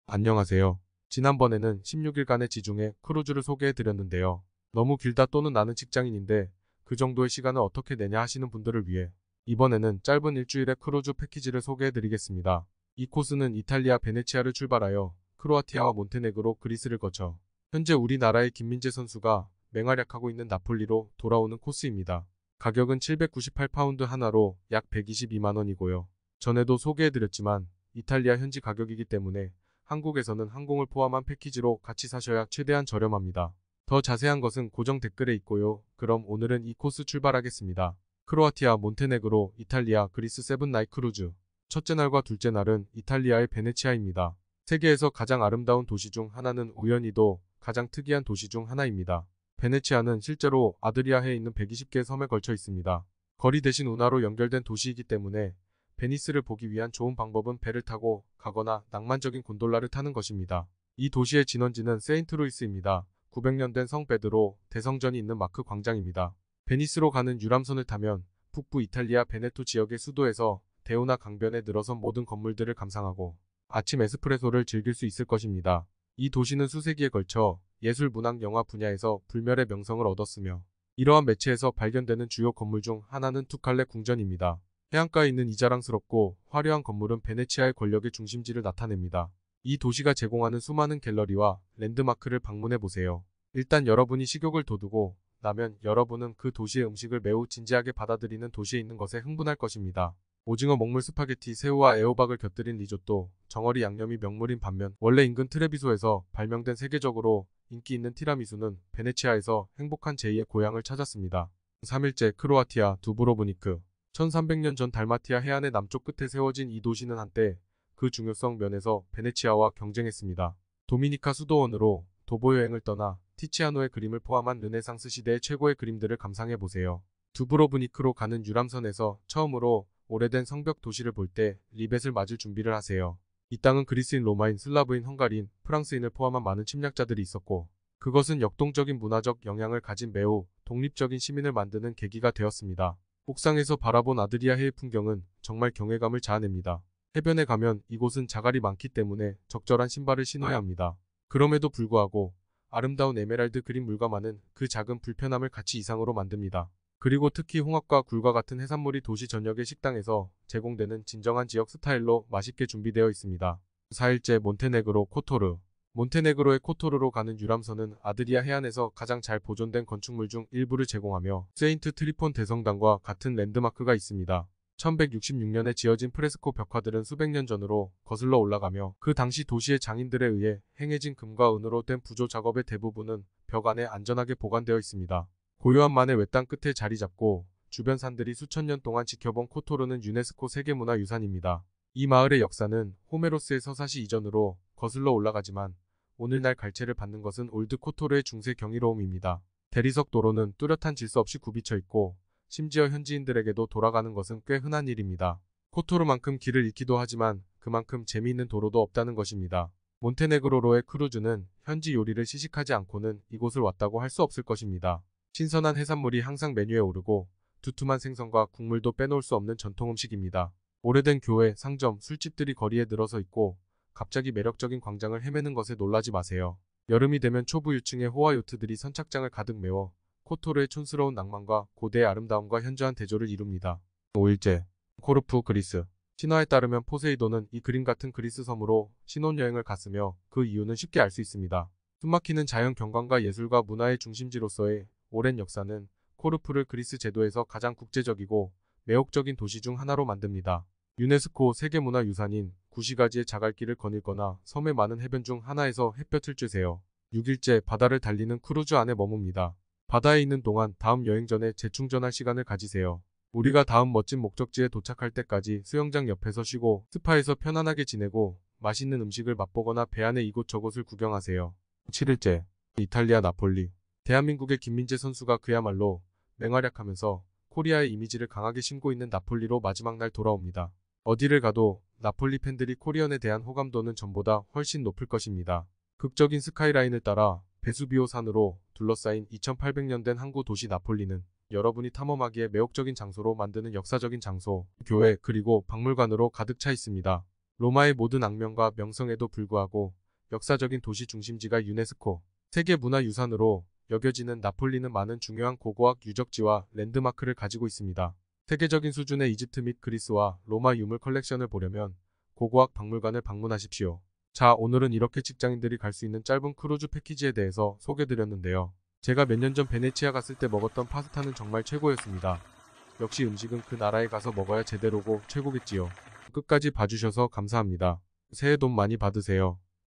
4개국 7일 유럽 크루즈 여행, 유럽은 처음이지? 크로아티아, 몬테네그로, 그리스, 이탈리아, 휴가 내고 가는 직장인에게 딱 좋은 코스!
런던은 처음이지? 크로아티아, 몬테네그로, 그리스, 이탈리아를 저렴하지만 알차게 즐기는 4개국 7일 유럽 크루즈 여행, 휴가 때만 갈 수 있는 직장인에게 딱 맞는 크루즈 패키지

Life is only once! Whatever you do, let's live sweetly!
단 한 번뿐인 인생! 뭘 하던 달콤하게 살자! 남에게 피해주지 않고 내 마음대로 살기!